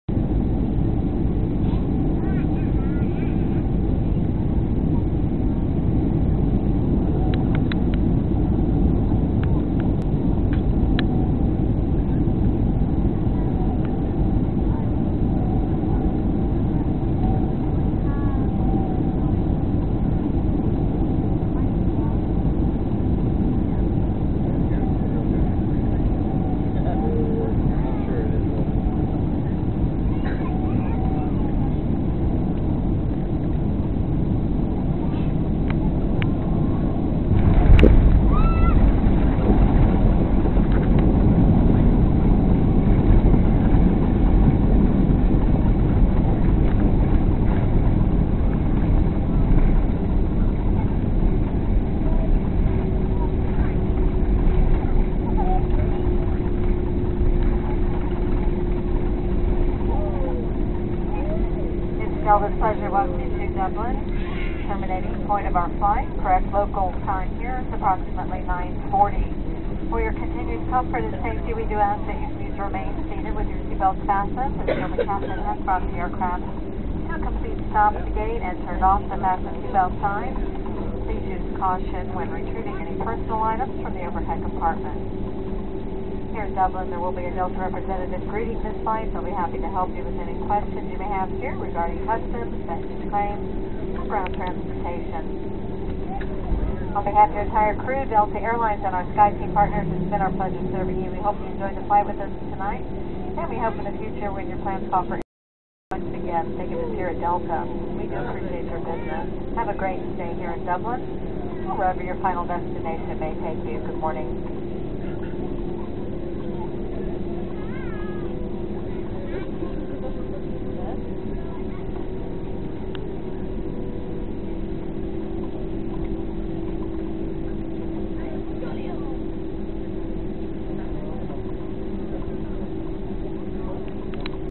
Landing at Dublin Airport
Onboard a Delta flight from Atlanta Georgia USA to Dublin on a boeing 767